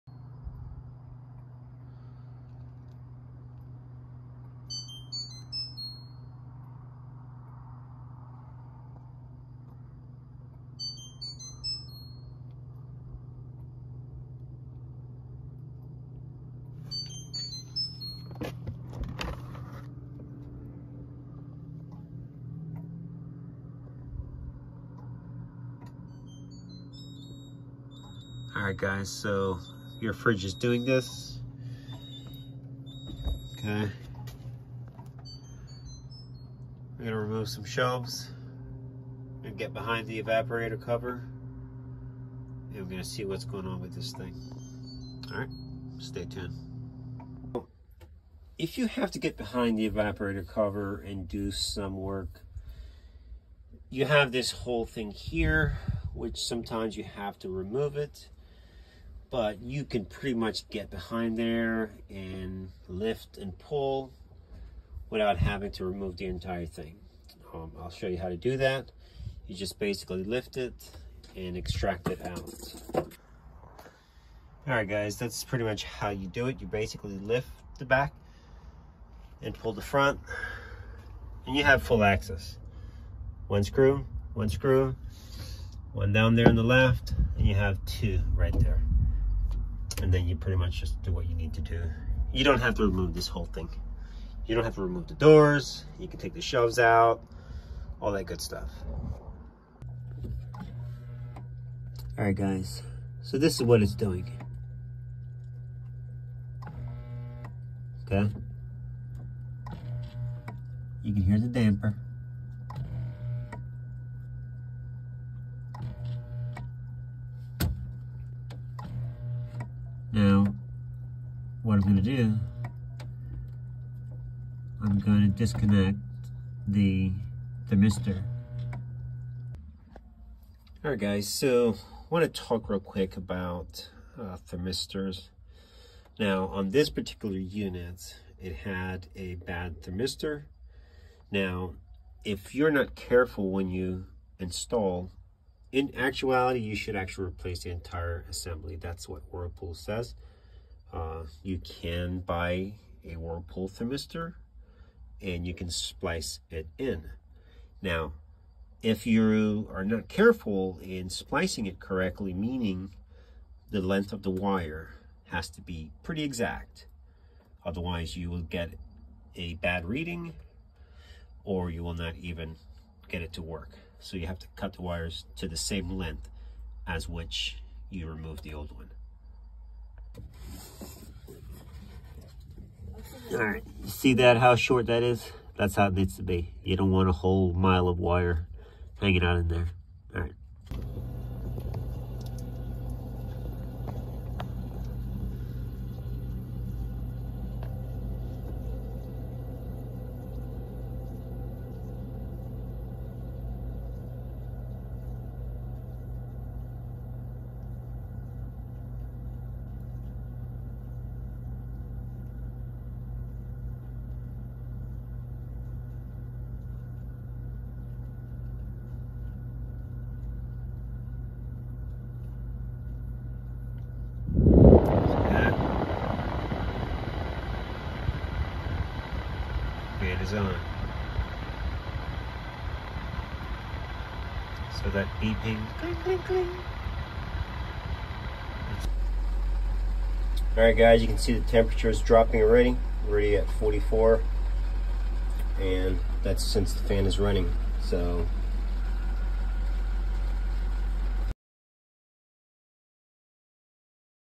Whirlpool Refrigerator Fresh Food Not cold| WRF993F 1FM00| Not Cold Enough | Beeping
WRF993F 1FM00 this the mode number of the refrigerator.  W10889841 COVER-EVAP. If you want to purchase the entire assembly that is the part number. It will have everything:fan, damper and thermistor. If you want to save and just replace the thermistor, here is a link.